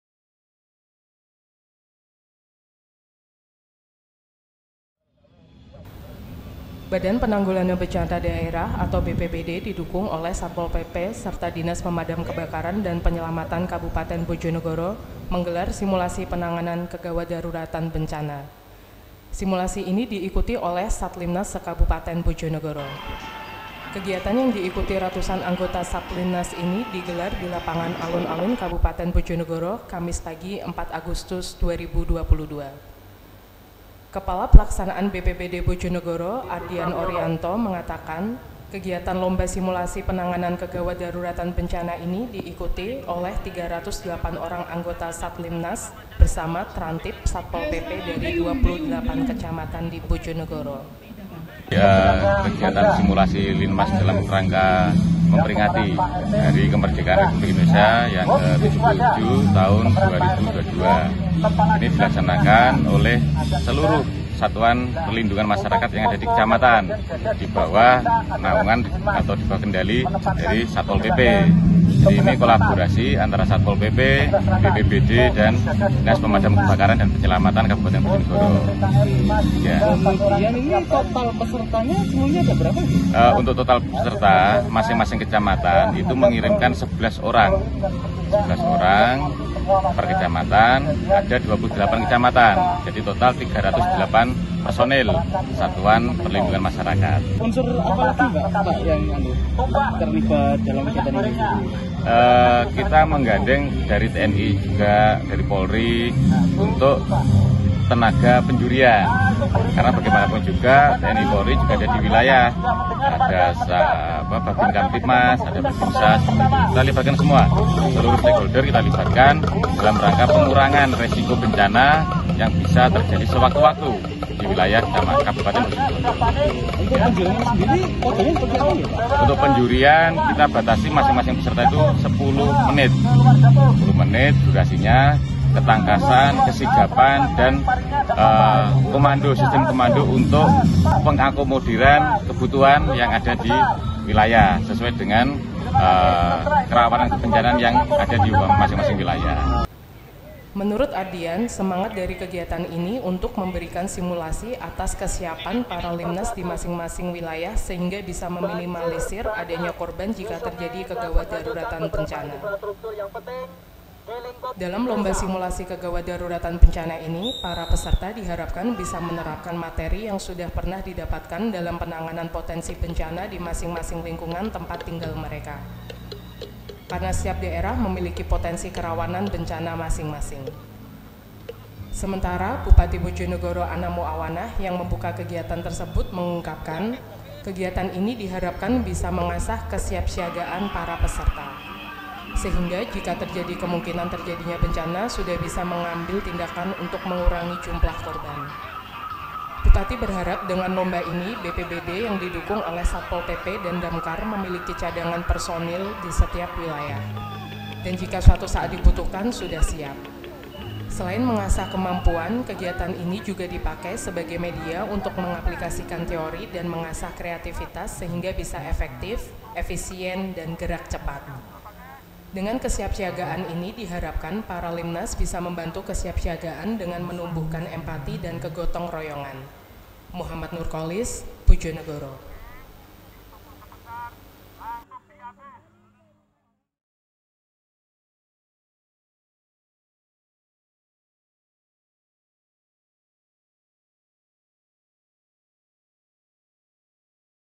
[BERITA RADAR] RATUSAN LINMAS SIMULASIKAN KEGAWATDARURATAN BENCANA
Badan Penanggulangan Bencana Daerah (BPBD) didukung oleh Satpol PP serta Dinas Pemadam Kebakaran dan Penyelamatan Kabupaten Bojonegoro menggelar lomba simulasi penanganan kegawatdaruratan bencana yang diikuti oleh Satlinmas se Kabupaten Bojonegoro.

Kegiatan yang diikuti ratusan anggota Satlinmas itu digelar di lapangan Alun-alun Kabupaten Bojonegoro, Kamis pagi 4 Agustus 2022.